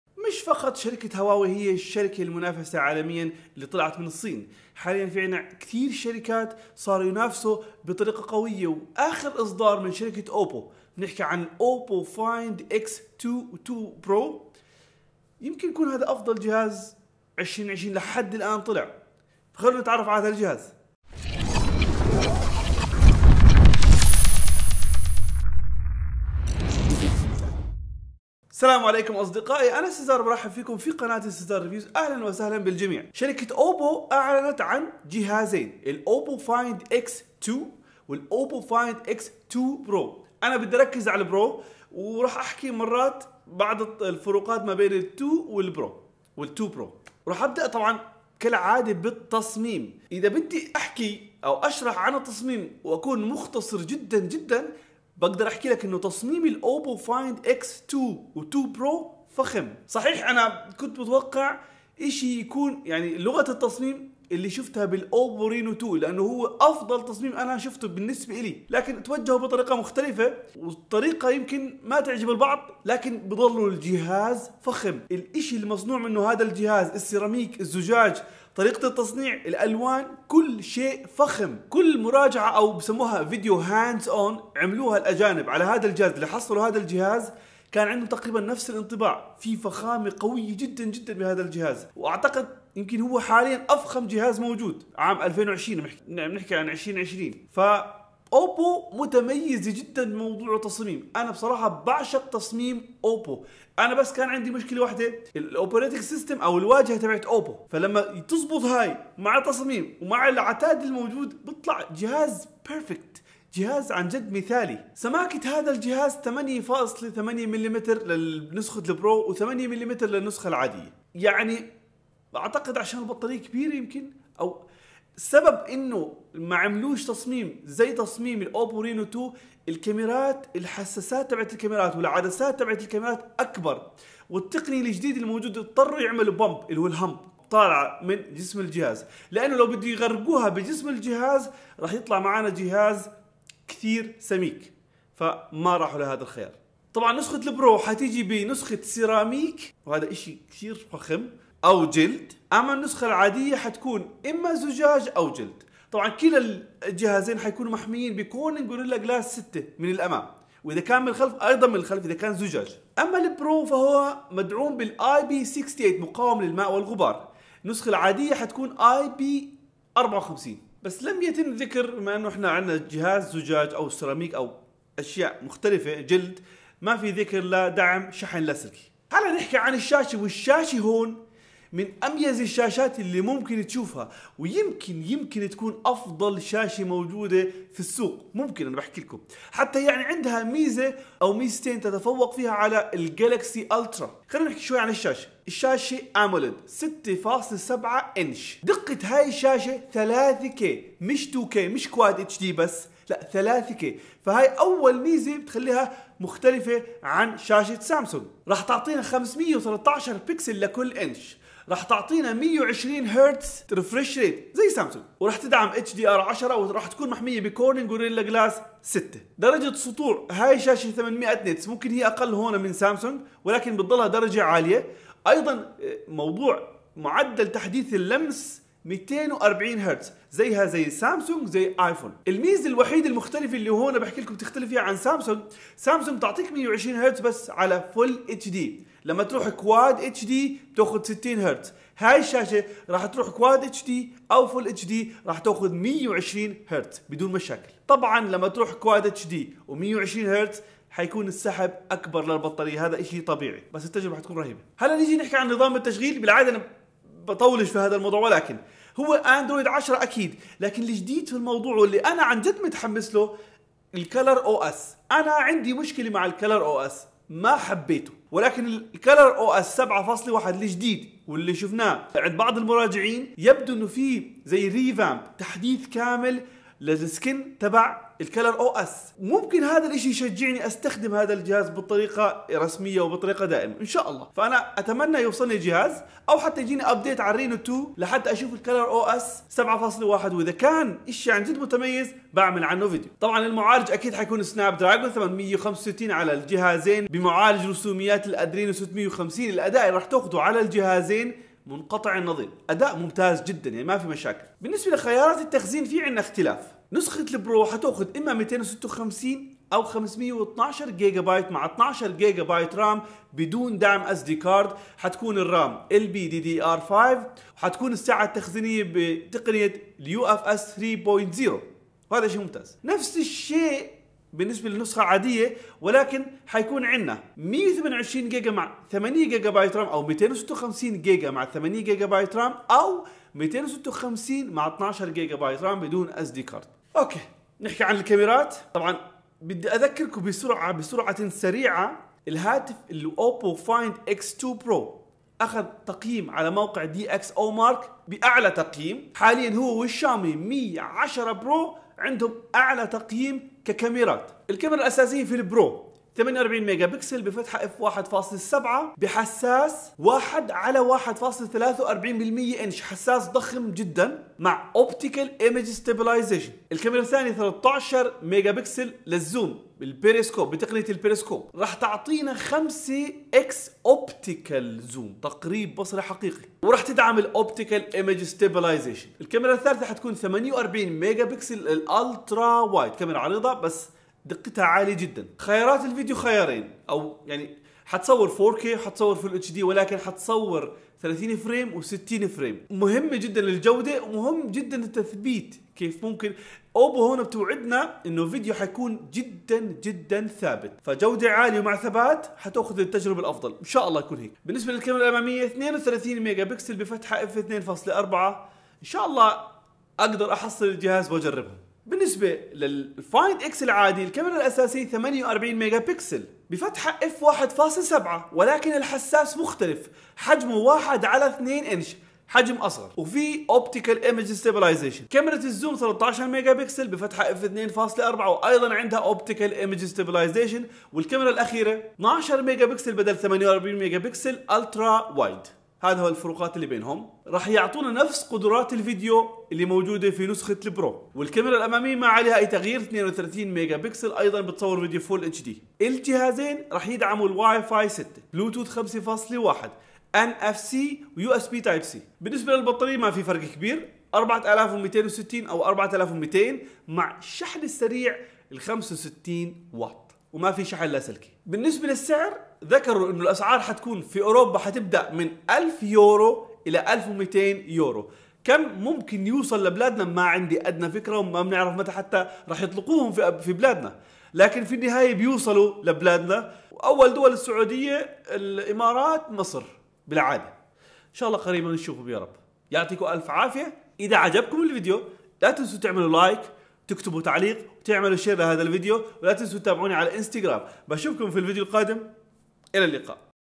هاتف oppo find x2 pro قد يكون أفضل هاتف اندرويد
أعلنت شركة أوبو عن أهم هواتفها
الفلاغ شيب الأقوى وهو الفايند إكس 2 برو
تعالوا نتعرف على مواصفات هذا الهاتف الفخم